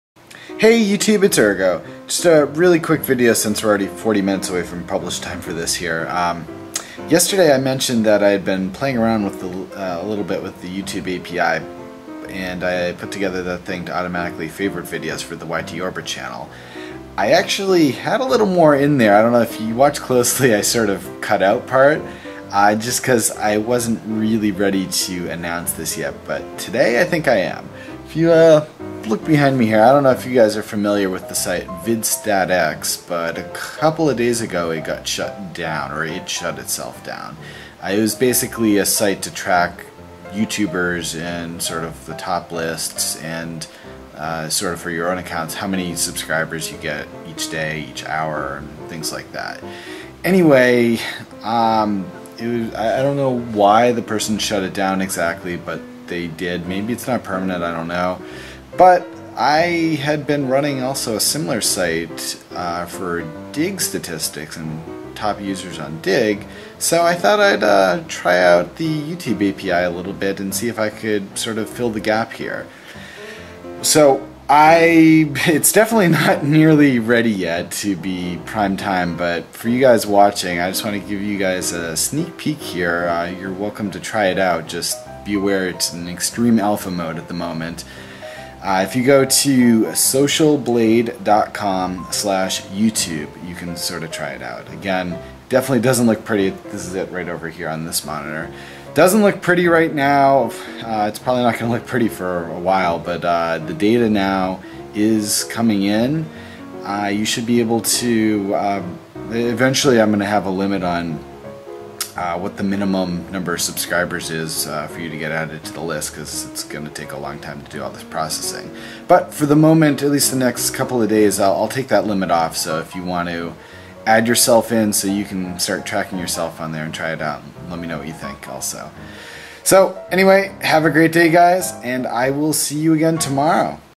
Social Blade YouTube Tracker - VidStatsX Alternative (YTO 286)
For a short while I've been using a site called vidstatx to track various statistics on my youtube account & just a couple of days ago it was shut down! So since I had already built a similar site for digg I decided to try my hand at the youtube api to create one for youtube :)

Anyway, it's far from done and in what I'll call 'extreme alpha mode' which means it may go down and have errors at any time but if you want to try it out by all means do so.  Just go to the site, plug in your username and wait a few hours and it will have stats for your account. :)

Ender's Weight Today: 24.5 lbs

Urgo's YouTube Orbit - Daily Video 79 of 365 - July 21th, 2010